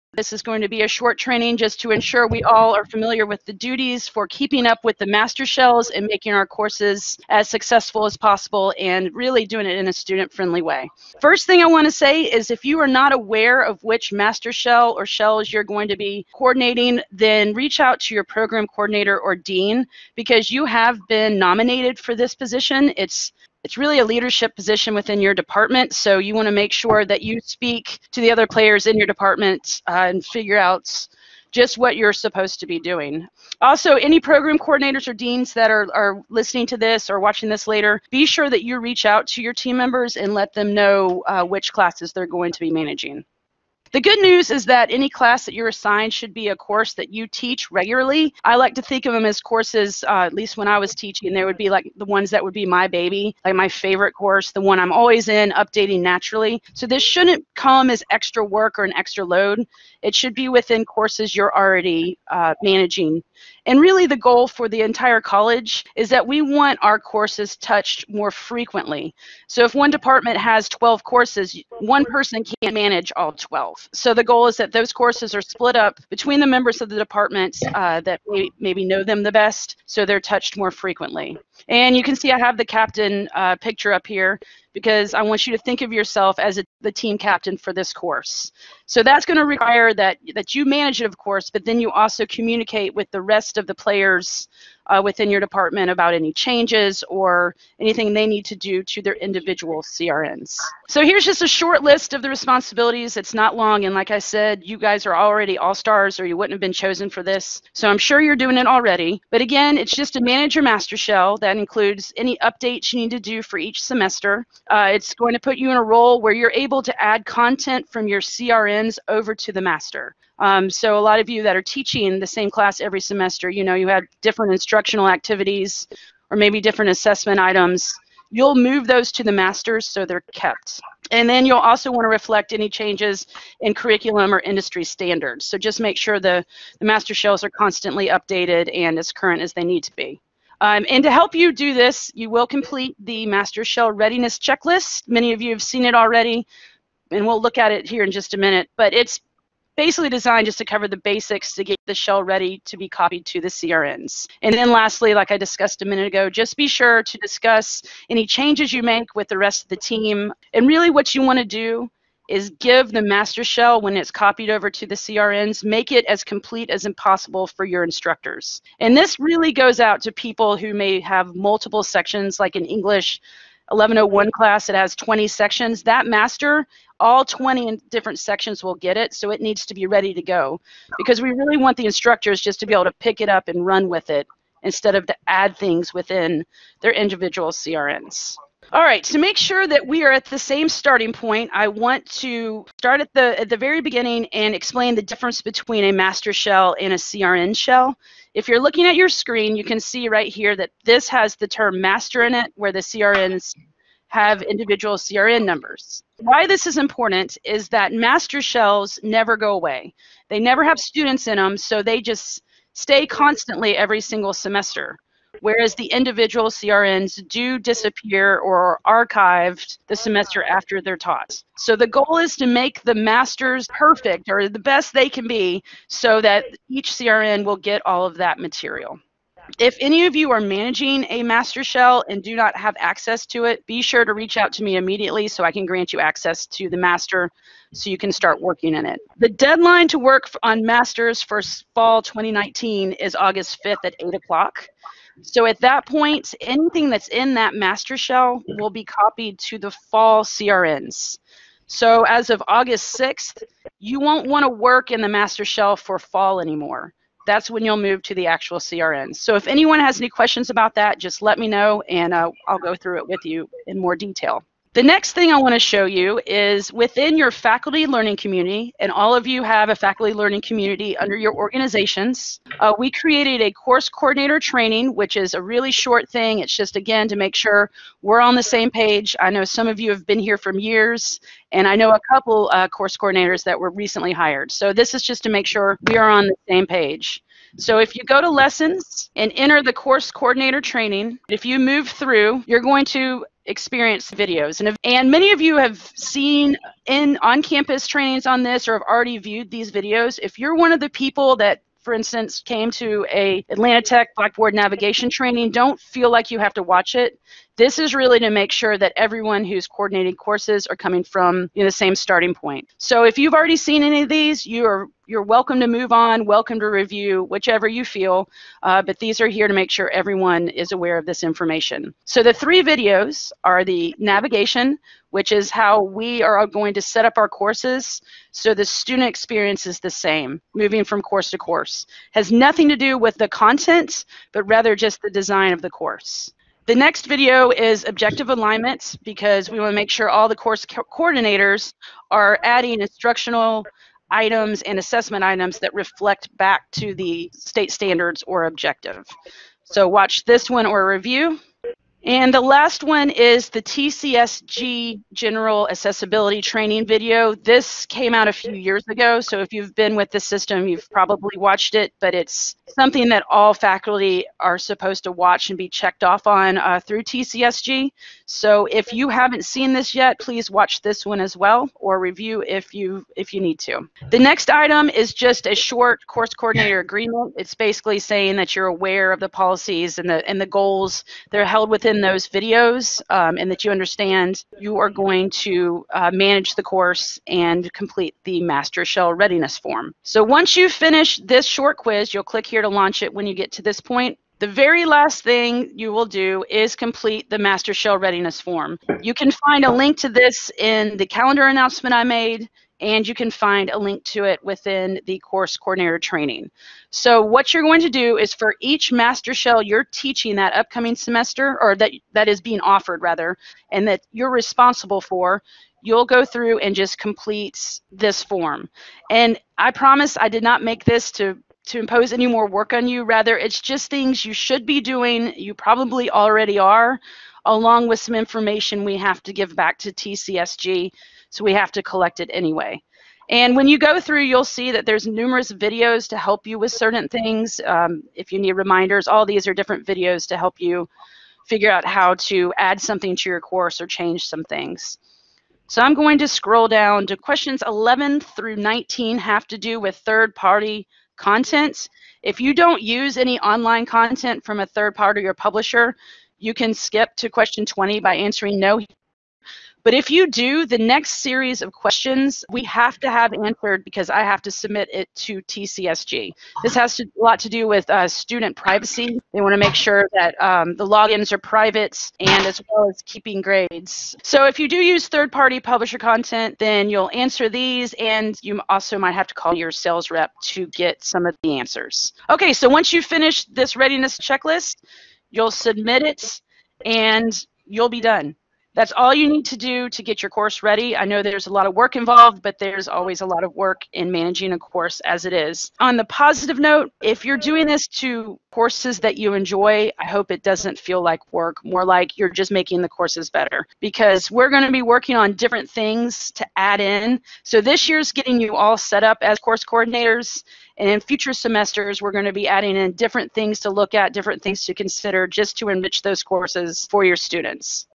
Course Coordinator Training
A training video for Course Coordinators at Atlanta Technical College.